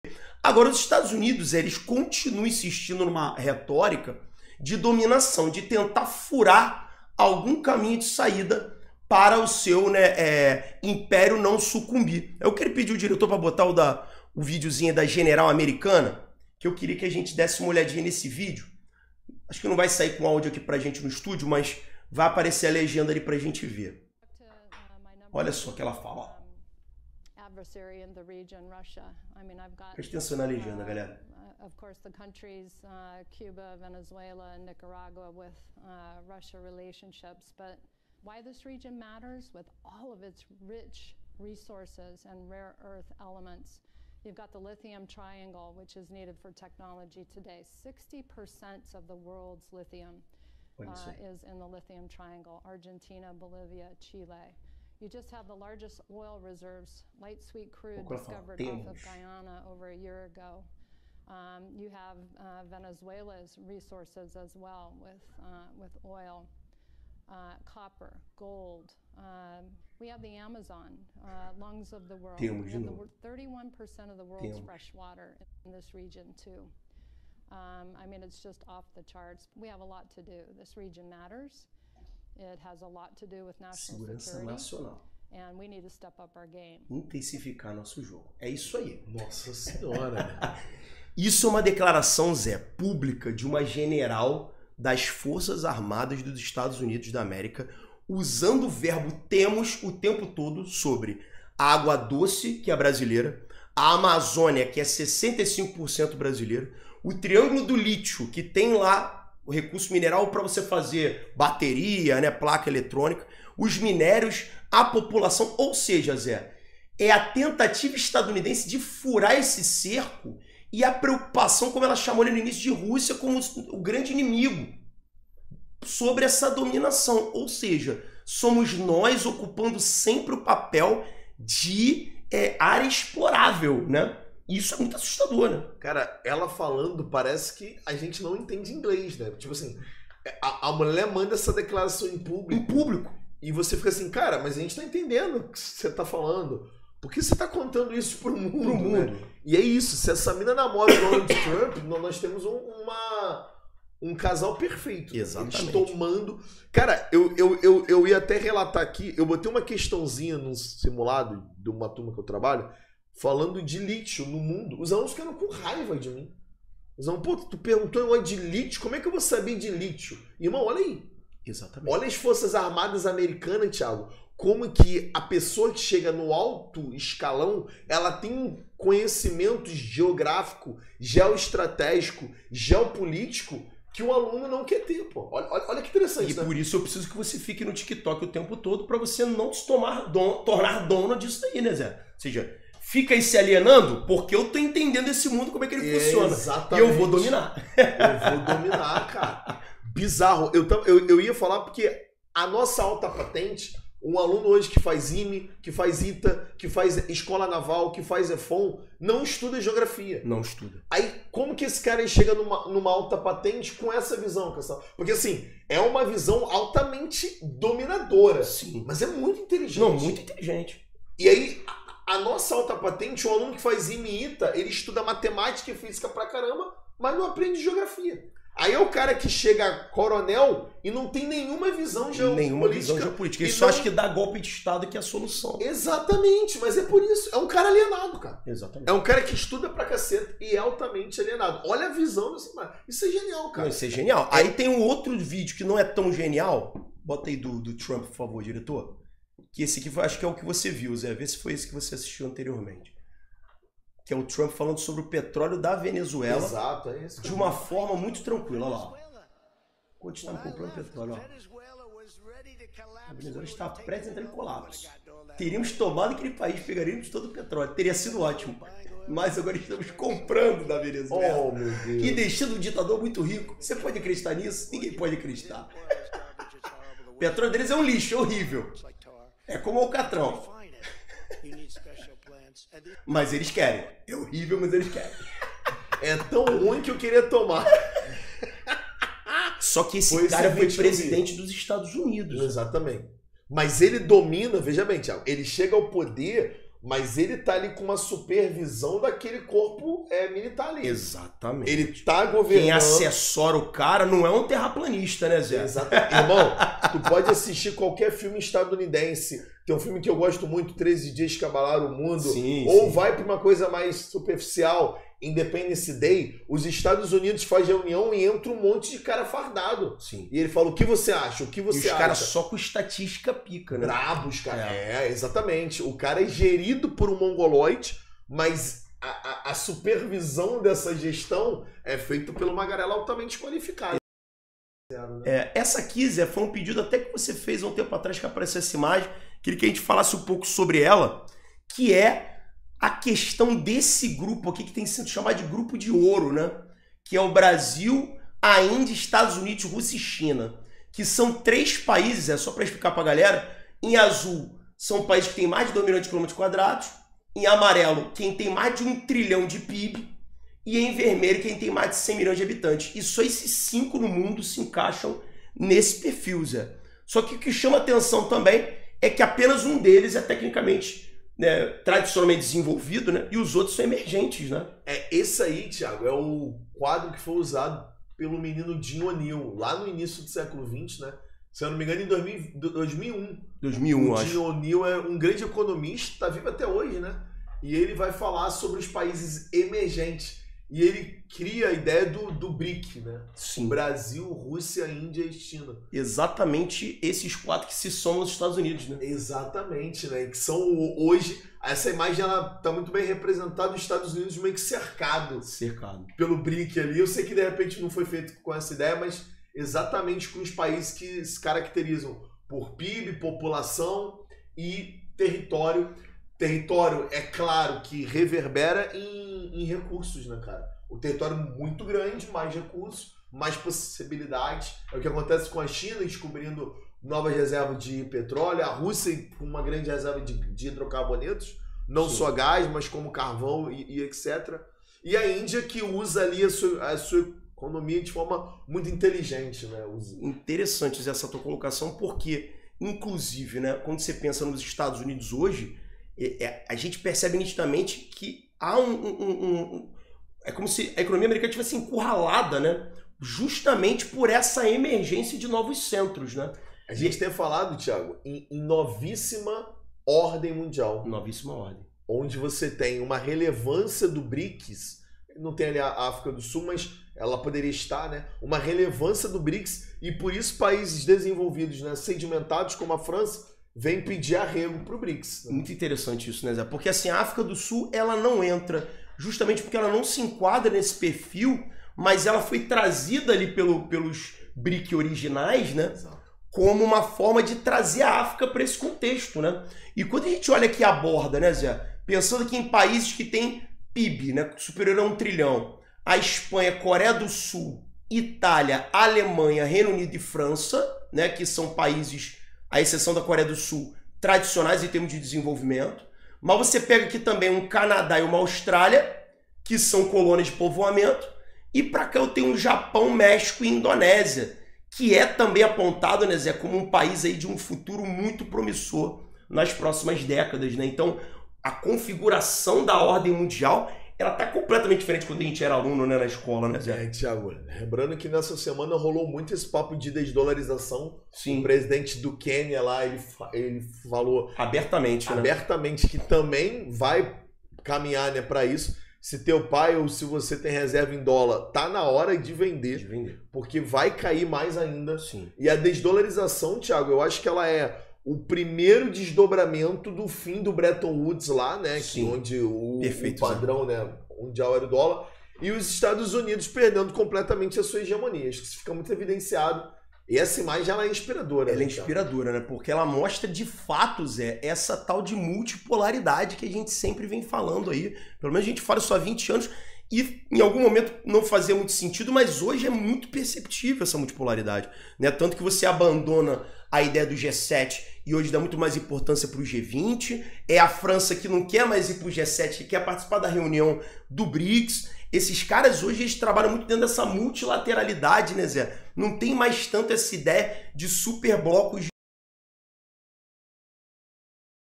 Os planos ambiciosos dos EUA para a América Latina
Mais um corte do sensacional Xtudo 🍔podcast, onde Zé Bonner e Thiago Feitosa analisam a ótica estadunidense sobre nossa região continental, assim como sua preocupação com uma maior autonomia e aproximação da China e Rússia. Quais são os objetivos dos EUA em nível regional? Venha descobrir!